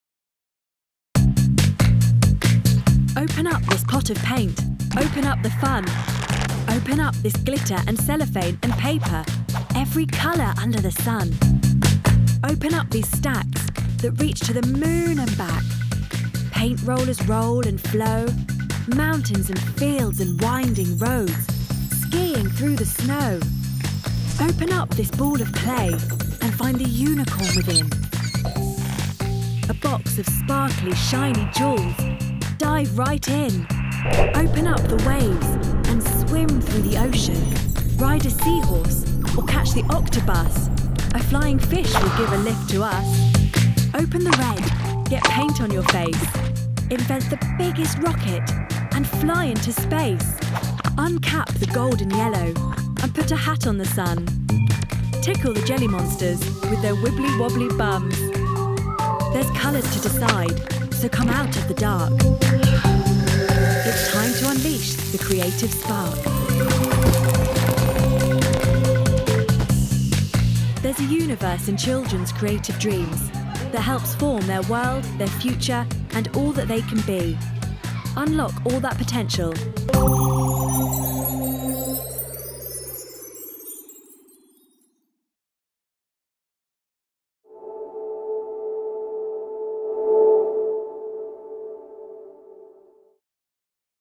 Amazing Art Activities for Children | The Creation Station
Art unlocks childrens' creative imaginations.
In this animation, paints, coloured papers, fabrics,modelling clays and fun produce fantastic creatures, places, machines and adventures in a child's imagination.
Amazing art and craft events, workshops and parties for 1 - 11 year olds at TheCreationStation.co.uk